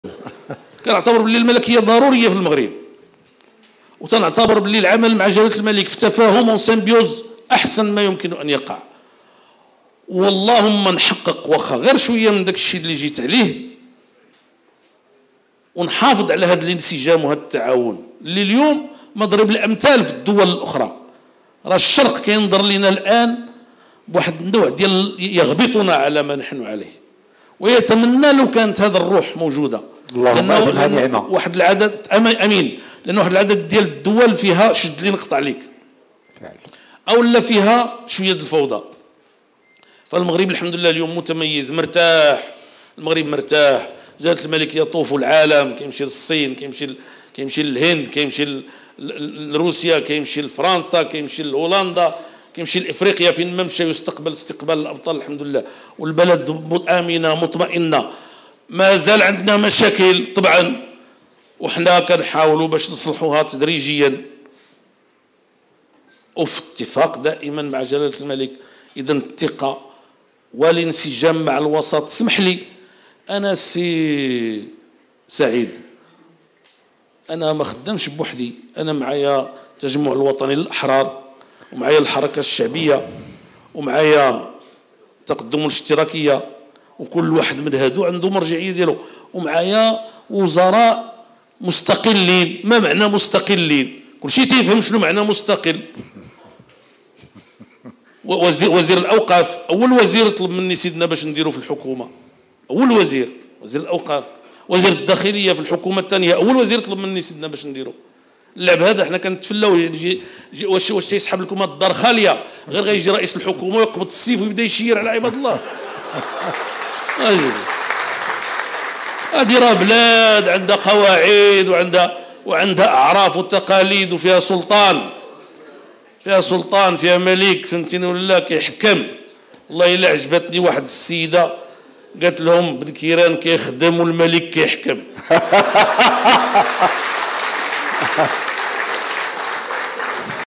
بنكيران والملكية : "واش سحاببليكوم رئيس الحكومة يجي يقبط السيف ويبقا بشير على عباد الله "
بنكيران والملكية
قال رئيس الحكومة عبد الاله بن كيران
واش سحاببليكوم رئيس الحكومة يجي يقبط السيف ويبقا بشير على عباد الله
وقال ايضا: " عجباتني واحد السيدة قالت الملك كيحكم وبنكيران كيخدم